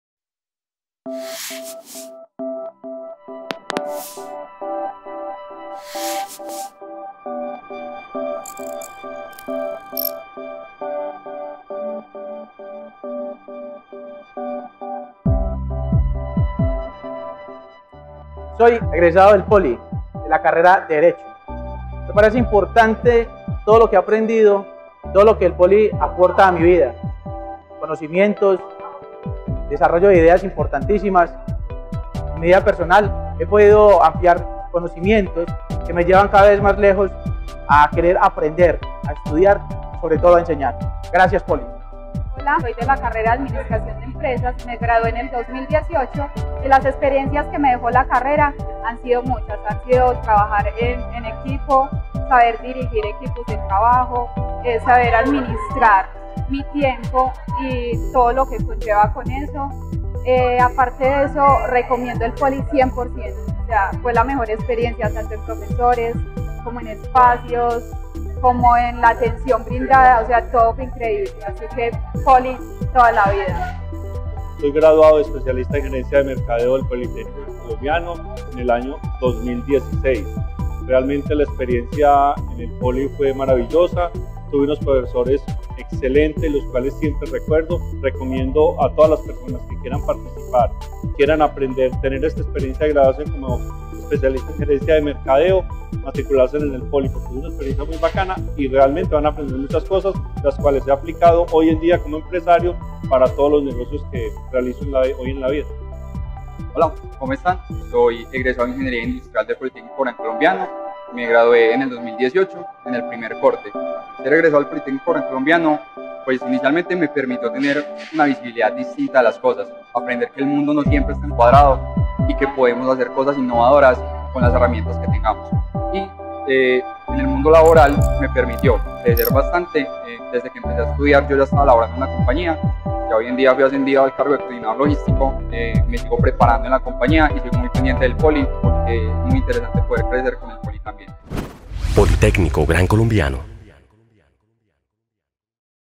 Encuentro de graduados en PoliMedellín
En PoliMedellín vivimos un encuentro de graduados donde nuestros estudiantes se encontraron con sus excompañeros y docentes.